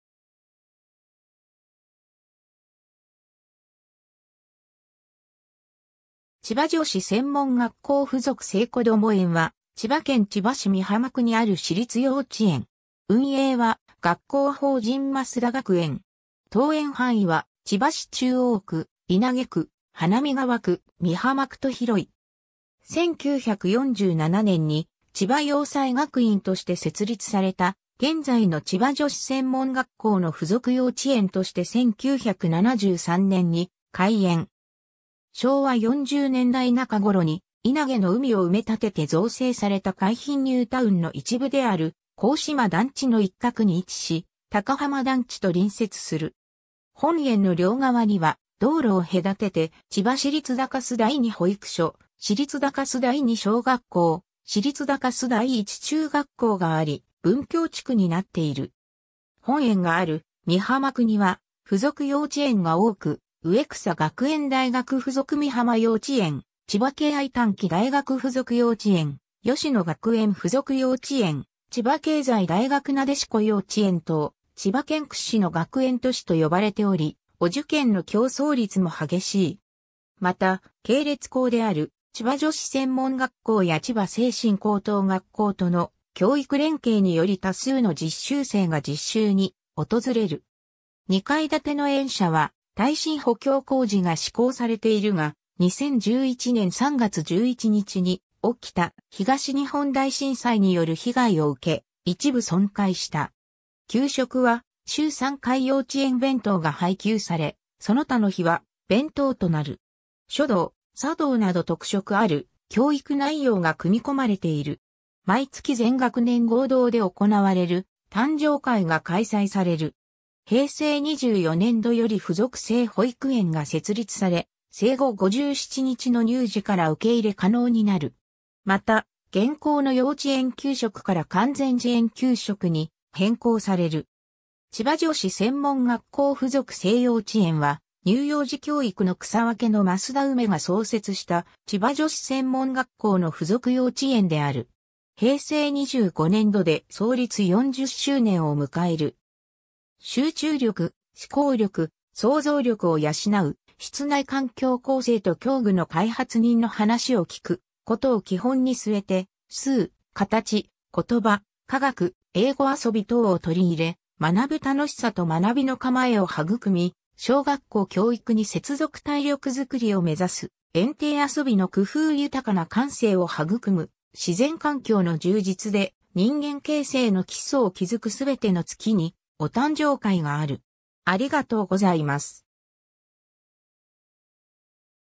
千葉女子専門学校附属聖幼稚園
千葉女子専門学校附属聖こども園（ちばじょしせんもんがっこうふぞくひじりこどもえん）は、千葉県千葉市美浜区にある私立幼稚園。
運営は学校法人増田学園。
 登園範囲は、千葉市中央区・稲毛区・花見川区・美浜区と広い。
 1947年に千葉洋裁学院として設立された現在の千葉女子専門学校の附属幼稚園として1973年に開園。
 昭和40年代中頃に稲毛の海を埋め立てて造成された海浜ニュータウンの一部である「高洲団地」の一角に位置し、高浜団地と隣接する。
本園の両側には、道路を隔てて千葉市立高洲第二保育所、市立高洲第二小学校、市立高洲第一中学校があり、文教地区になっている。
 本園がある美浜区には附属幼稚園が多く、植草学園大学附属美浜幼稚園、千葉敬愛短期大学附属幼稚園、芳野学園附属幼稚園、千葉経済大学なでしこ幼稚園（平成20年度に閉園）等、千葉県屈指の学園都市と呼ばれており、お受験の競争率も激しい。
 また、系列校である千葉女子専門学校や千葉聖心高等学校との教育連携により多数の実習生が実習に訪れる。
 2階建ての園舎は耐震補強工事が施工されているが、2011年3月11日に起きた東日本大震災による被害を受け、一部損壊した（改修済）。
 給食は週3回幼稚園弁当が配給され、その他の日は弁当となる。
書道・茶道など特色ある教育内容が組み込まれている。
毎月全学年合同で行われる誕生会が開催される。
 平成24年度より附属聖保育園が設立され生後57日の乳児から受け入れ可能になる。
また、現行の幼稚園給食から完全自園給食に変更される。
 千葉女子専門学校附属聖幼稚園は、乳幼児教育の草分けの増田うめが創設した千葉女子専門学校の附属幼稚園である。
平成25年度で創立40周年を迎える。
 集中力、思考力、想像力を養う室内環境構成と教具の開発 人の話を聞くことを基本に据えて、数、形、言葉、科学、英語遊び等を取り入れ、学ぶ楽しさと学びの構えを育み小学校教育に接続 体力作りを目指す園庭遊びの工夫 豊かな感性を育む自然環境の充実で人間形成の基礎を築く ※全ての月にお誕生会がある。